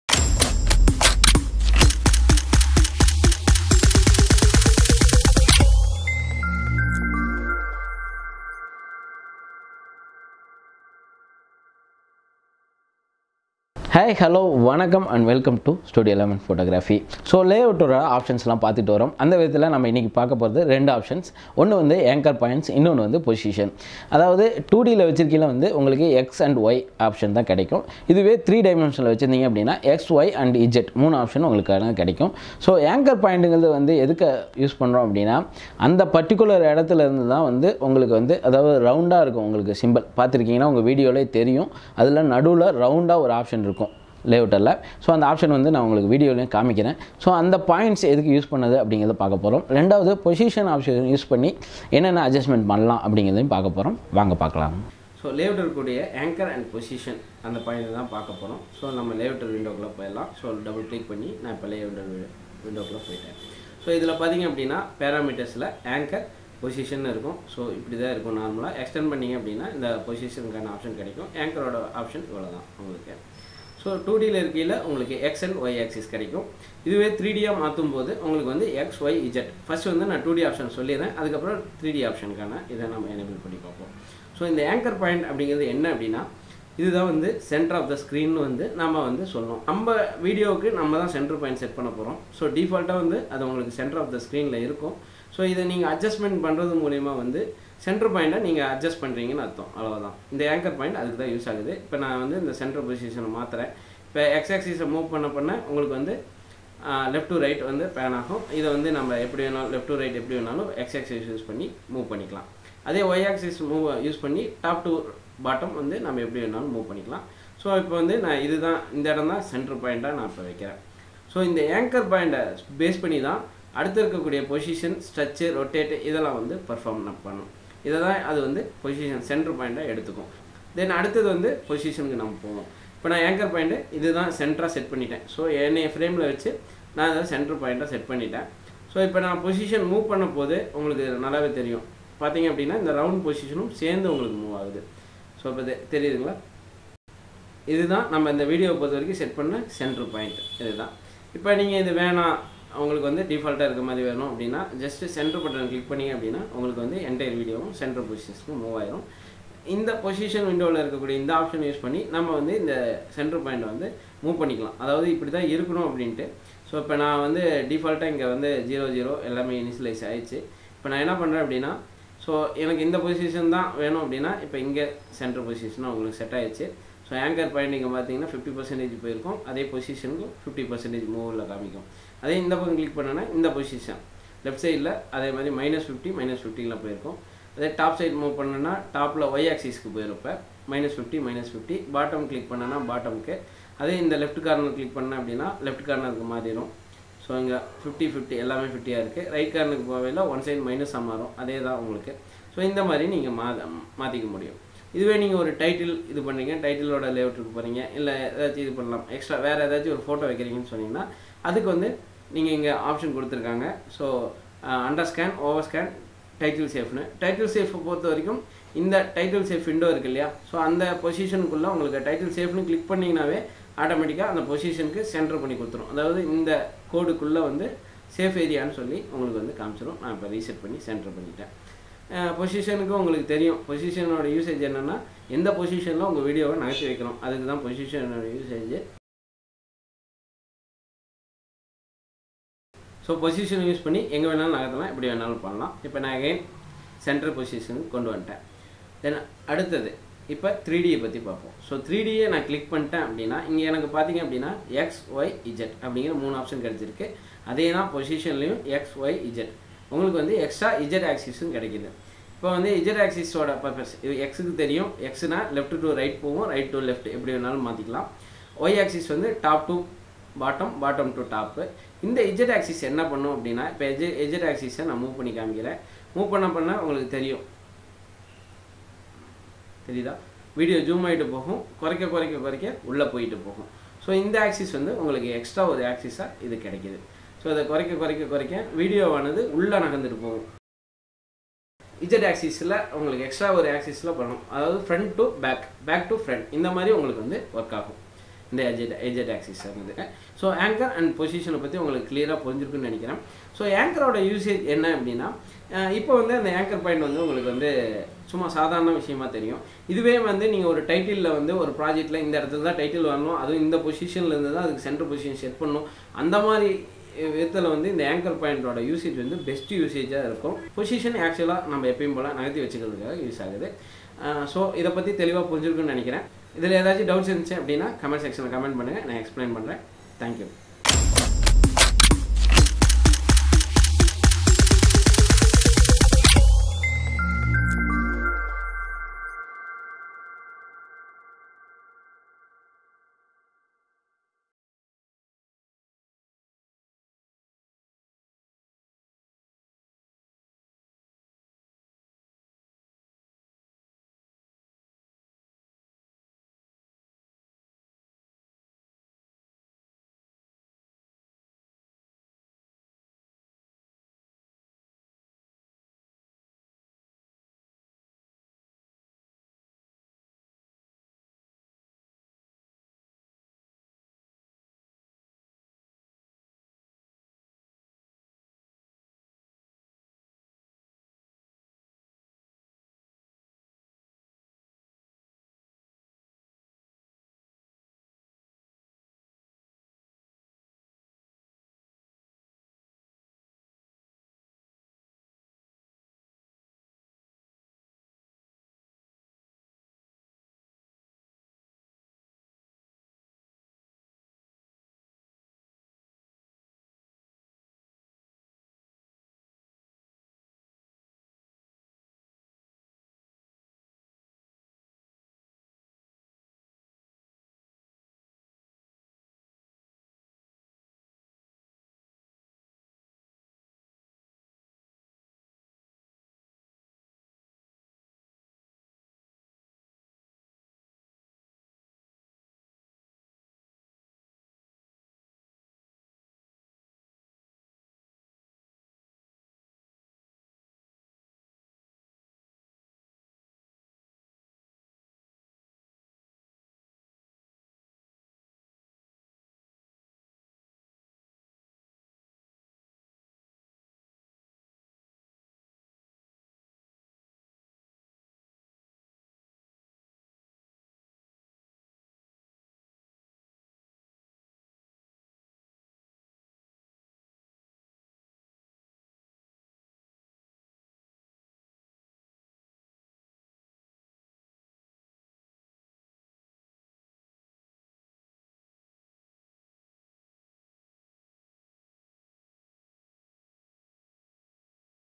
47 | Anchor & Position Options in Layouter | Usage of Anchor Point | Edius Tutorials in Tamil
47 | Anchor & Position Options in Layouter | Usage of Anchor Point | Edius Tutorials in Tamil

EDIUS பற்றி தெரிந்த தகவல் தெரியாத தகவல் கற்பித்தல் மற்றும் கற்றலுக்காக உருவாக்கப்பட்டது.

குழுவில் இணைய  இந்த linkயை பயன்படுத்தவும்

Share செய்து பயன்பெறவும்